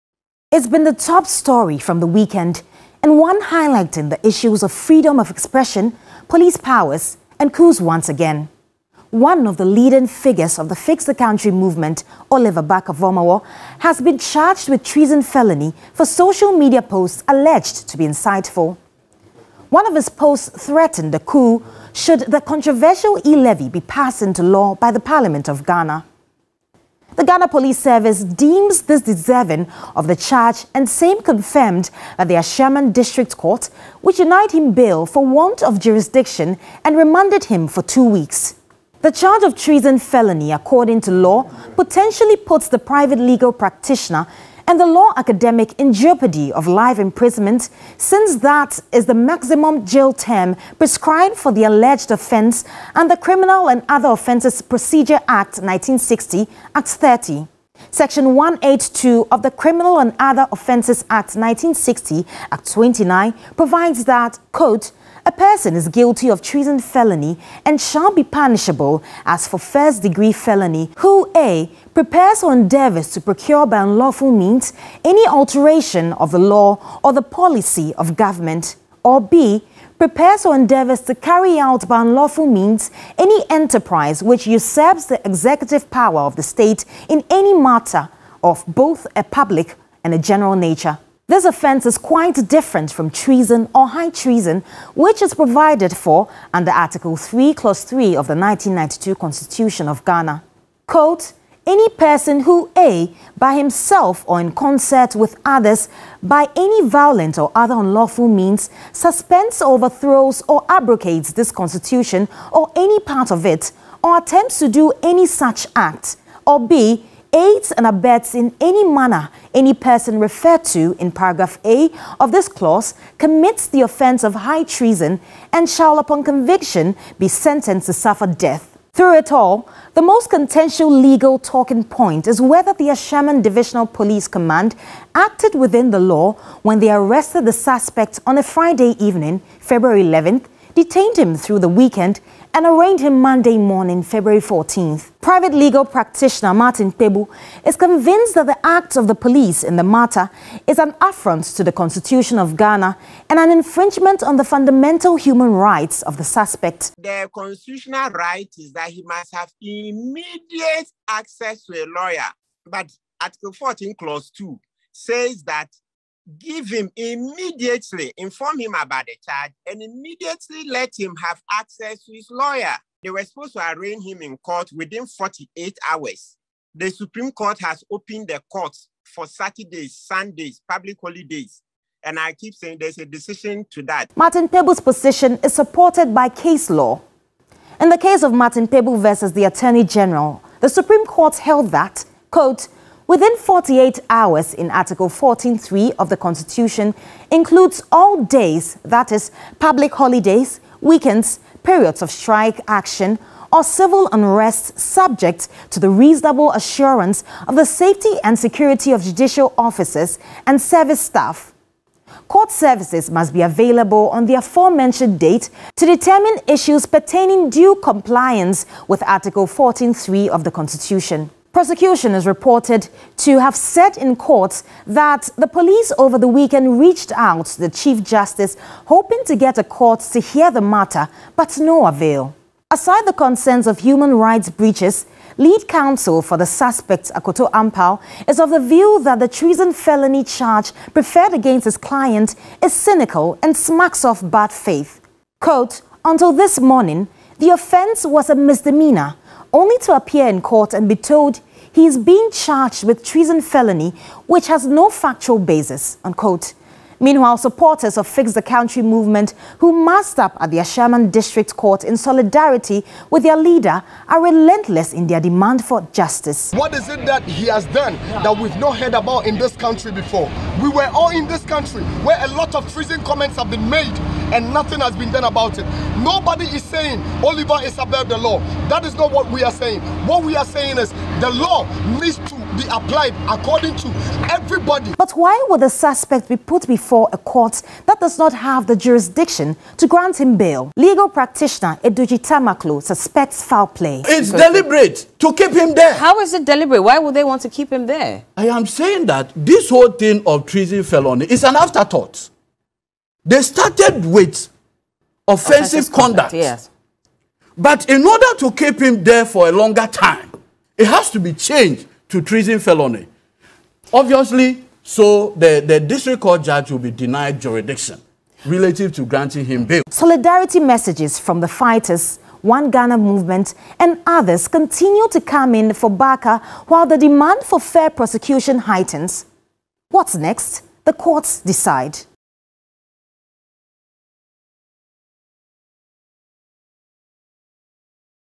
Oliver Barker-Vormawor fights back his detention -Here is a break down of the key issues Judith
A fast-developing running story many struggle to wrap their head around and may even get complicated in the coming days as the convenor for the FixTheCountry Movement, Oliver Barker-Vormawor fights back his detention.

Here is a break down of the key issues so far by Judith Awortwi-Tandoh.

Producer by P. D Wedam and edited by Moses Yeboah.